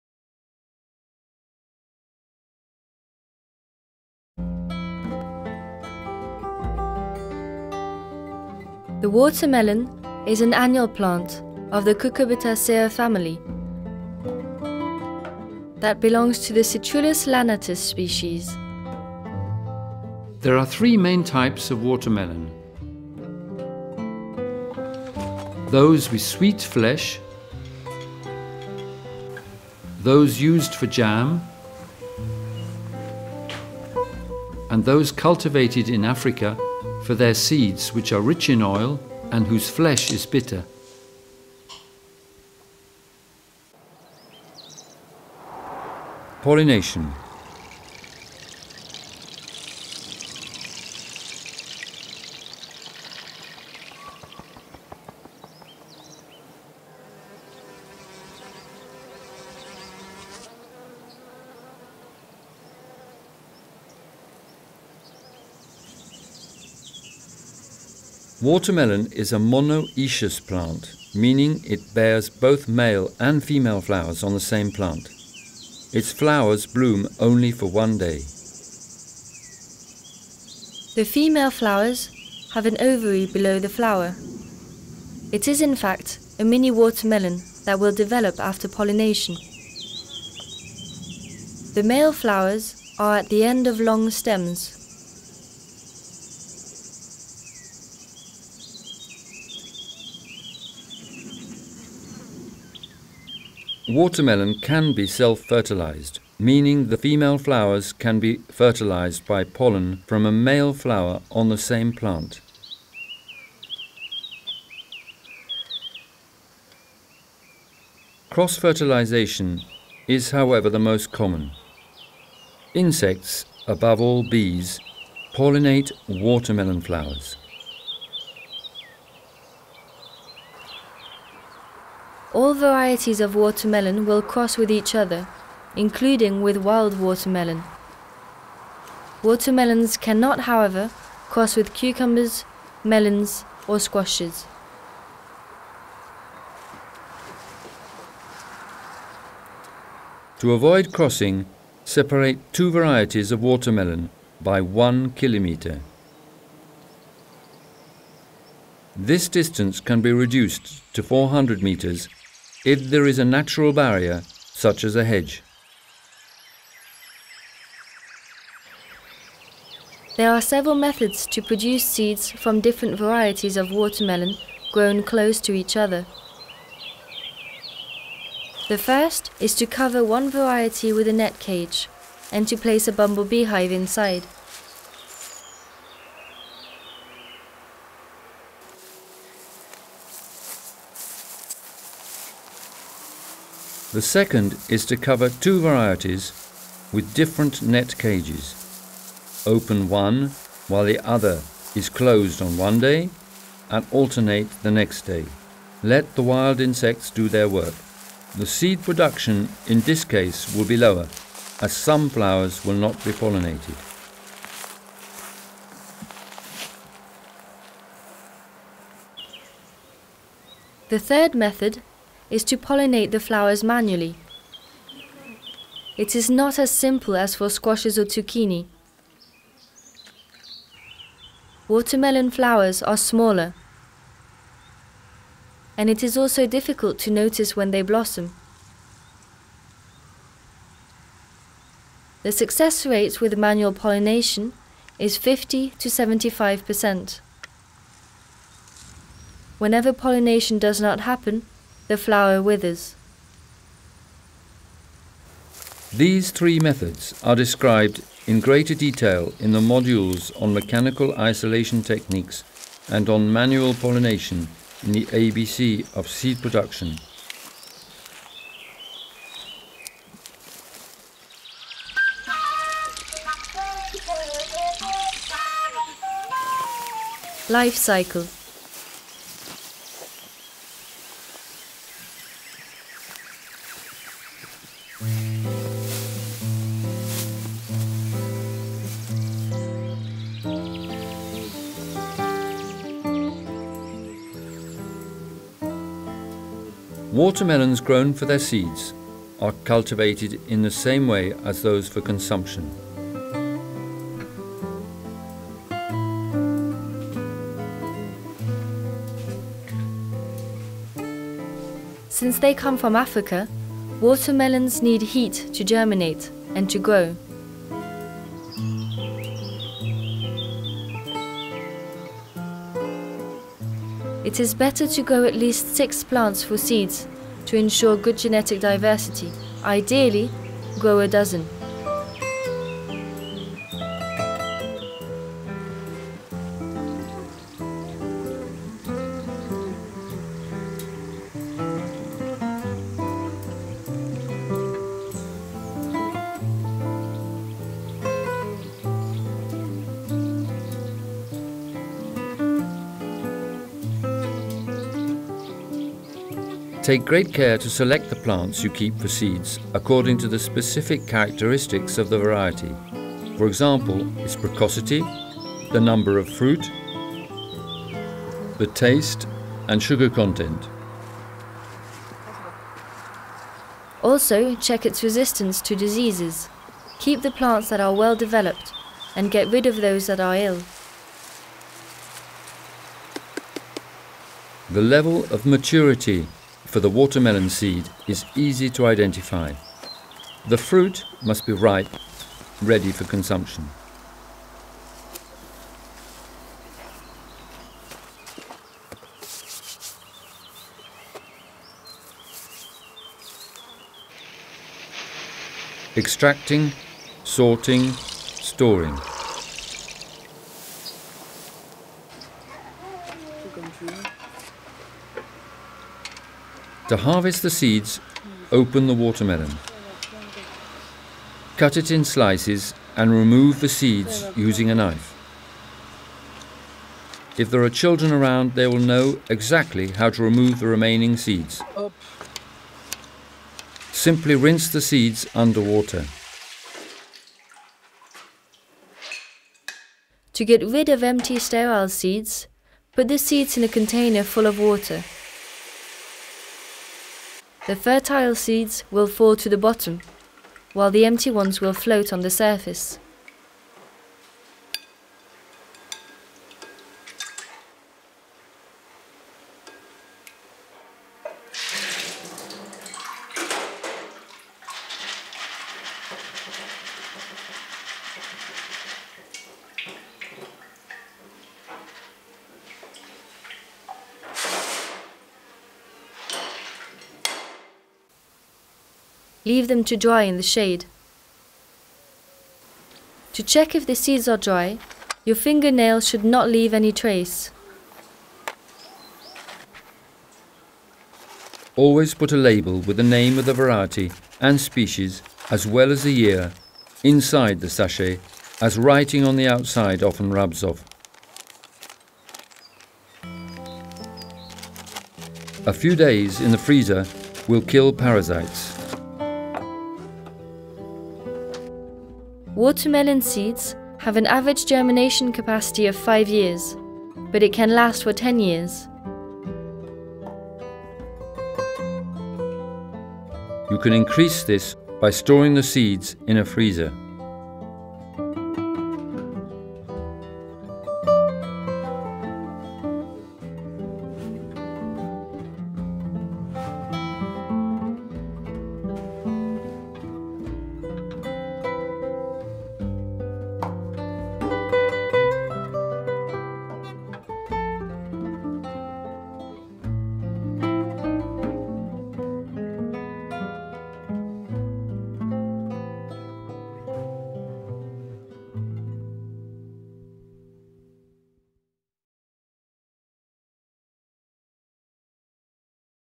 How to save watermelon seeds
In this video, learn how to save your own watermelon seeds step by step.

Chapters:
00:06 Botanical classification and diversity
00:45 Pollination
04:32 Life cycle
06:47 Extraction, cleaning & storage of seeds